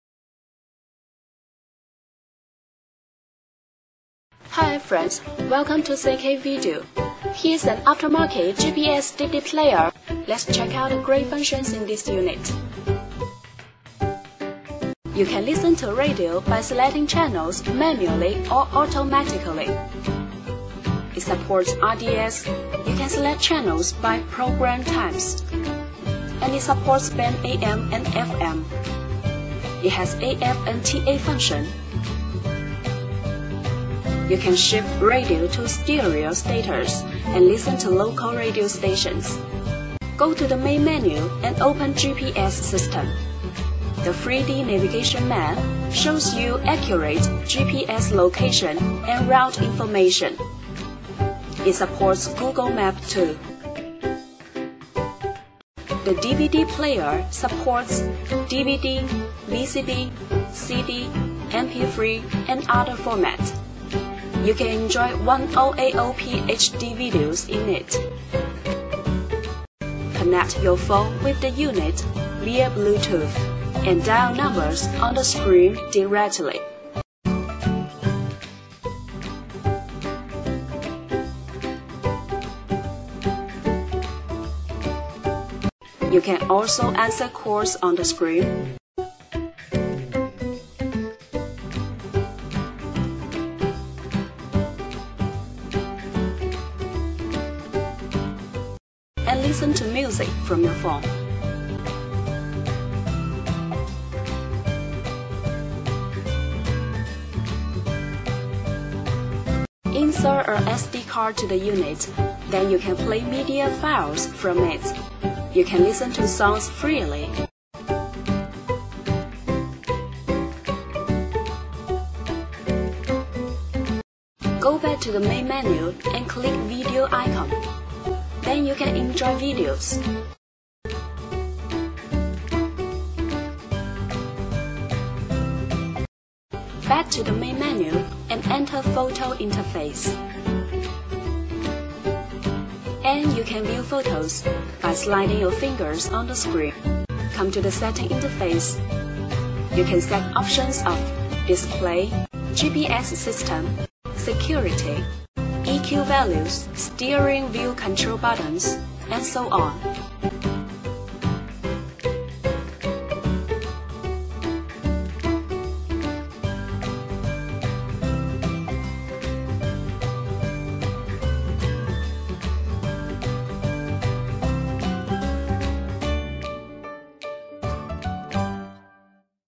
All in one 2010-2013 Opel Astra aftermarket radio bluetooth dvd navigation system support ODB2
Plug and play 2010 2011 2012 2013 Opel Astra aftermarket radio bluetooth dvd navigation system support OBD2 Backup Camera. Functions:High Speed 3G&WiFi Internet, App download, 3D Navigation, Radio Tuner, Bluetooth, CD DVD player, File Management, HD 1080P video, Steering wheel control, 32GB USB/SD Connection. Send a picture of your car's dashboard with the year and model enclosed to our email now, and our pre-sale service team will check if this system works with your car.Wish you like it.